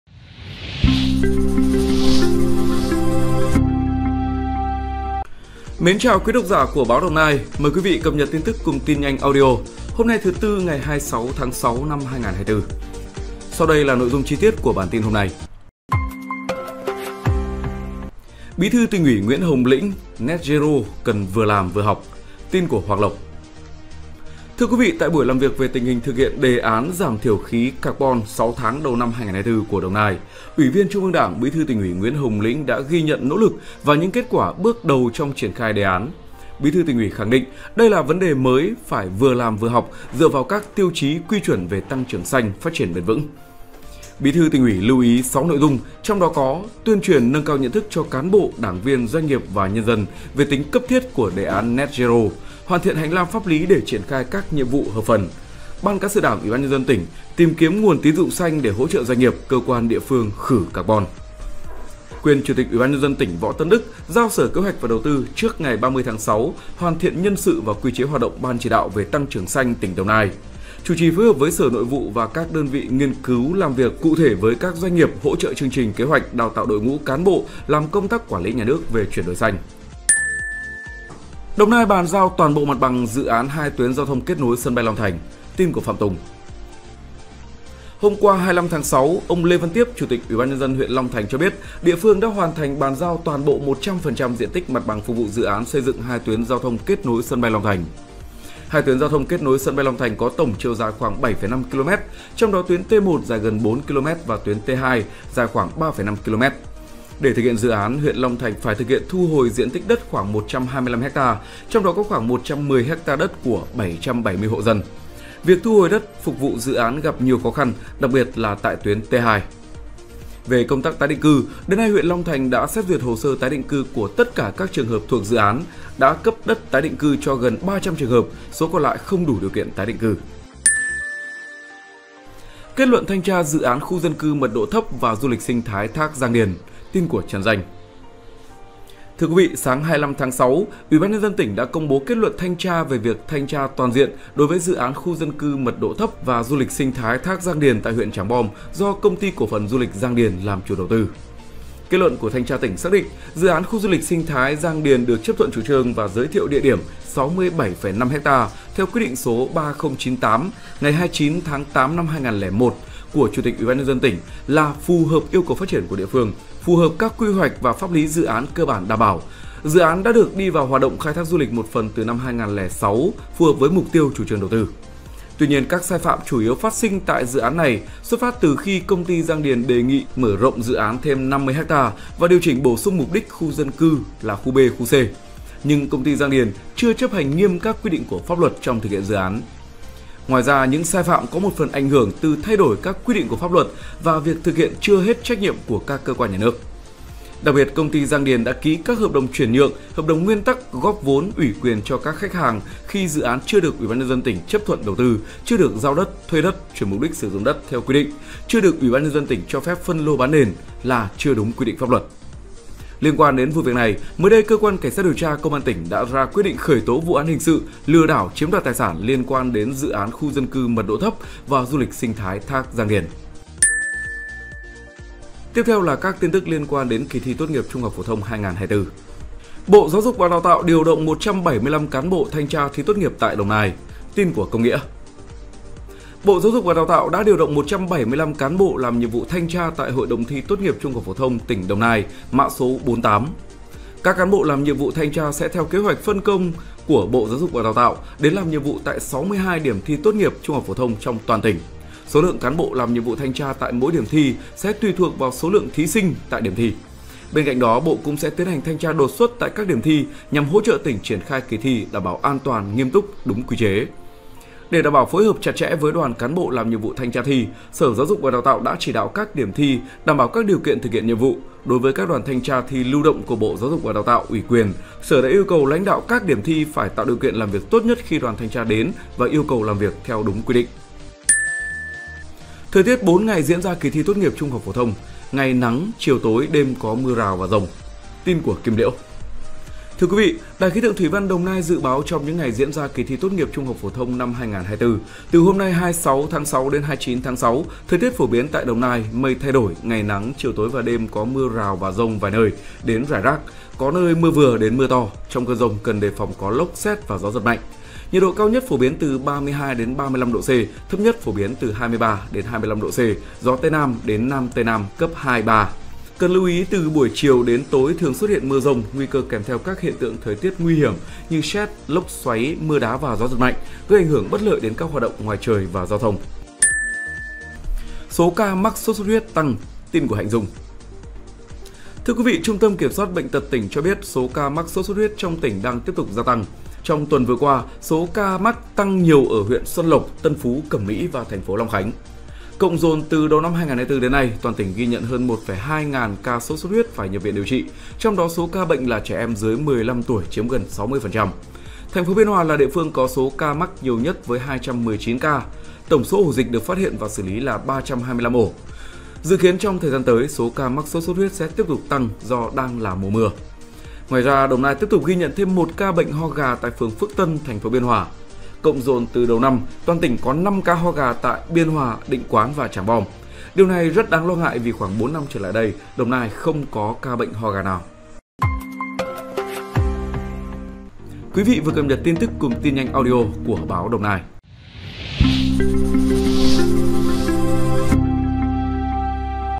PodCast 26-6-2024
Tin nhanh audio 26-6: Đồng Nai bàn giao toàn bộ mặt bằng dự án 2 tuyến giao thông kết nối Sân bay Long Thành; Kết luận thanh tra Dự án Khu dân cư mật độ thấp và du lịch sinh thái thác Giang Điền...